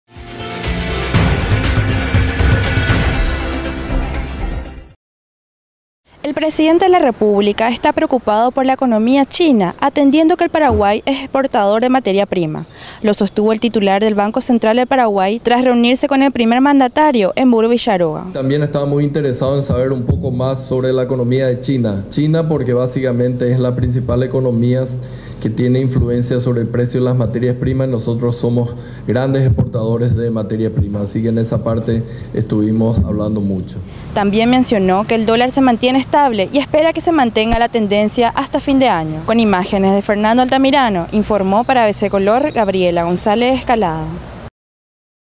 Preocupa impacto de “bajón” chino
El titular del BCP se refirió al impacto de la debilidad de la economía china en Paraguay, lo que preocupa al Gobierno.